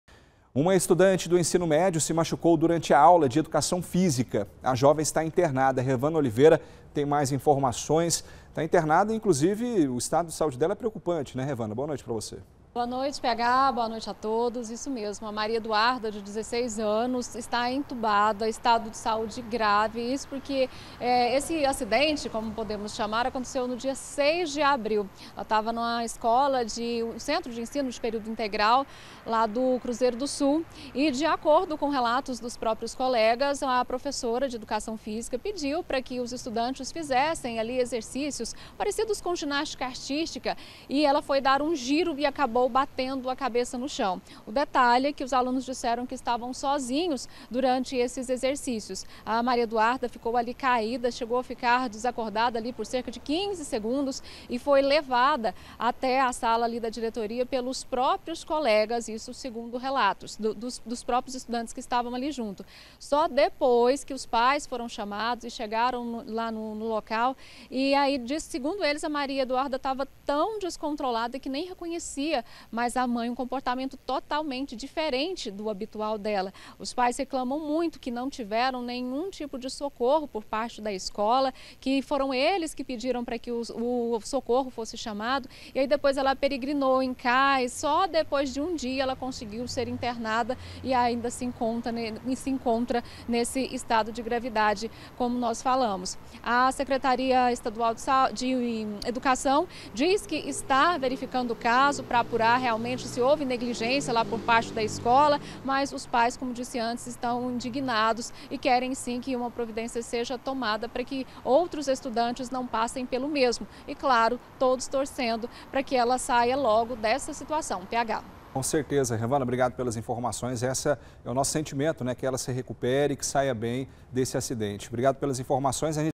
ESTADO GRAVE: JOVEM SE ACIDENTA DURANTE AULA DE EDUCAÇÃO FÍSICA
UMA ESTUDANTE DO ENSINO MÉDIO SE MACHUCOU DURANTE A AULA DE EDUCAÇÃO FISICA. JOVEM ESTÁ INTERNADA.